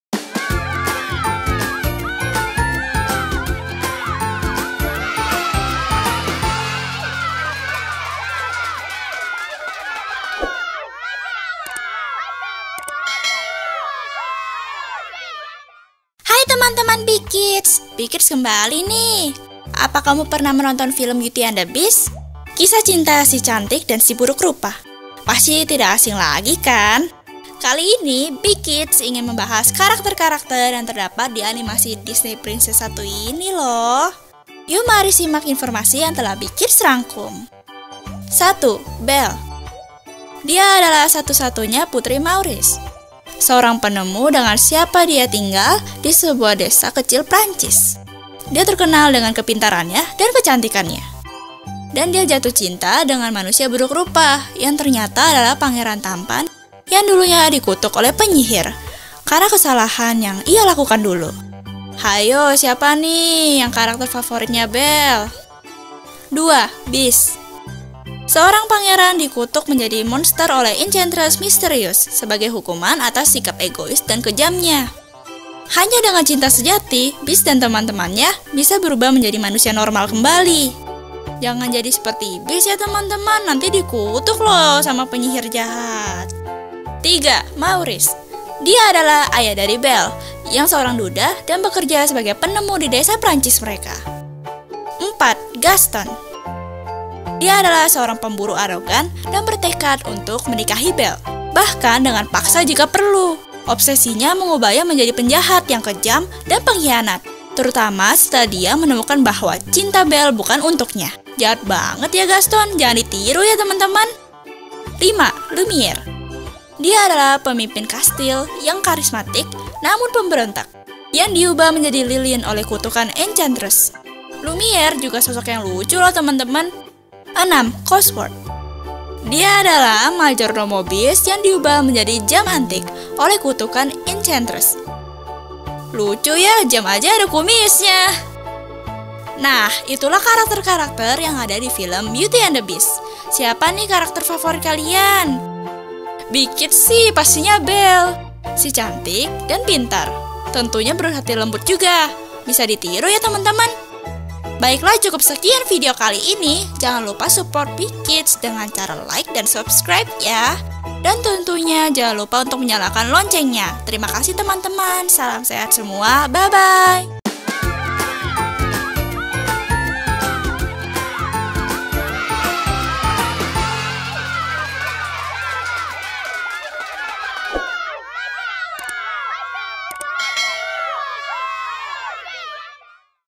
Nama - Nama Karakter Beauty and The Beast ( Video Anak Pintar ) - Bie KIDS
Karakter Karakter  Beauty and The Beast berbeda - beda lohh...
Sahabat Bie Kids harus tau nih siapa saja karakter karakter Beauty and The Beast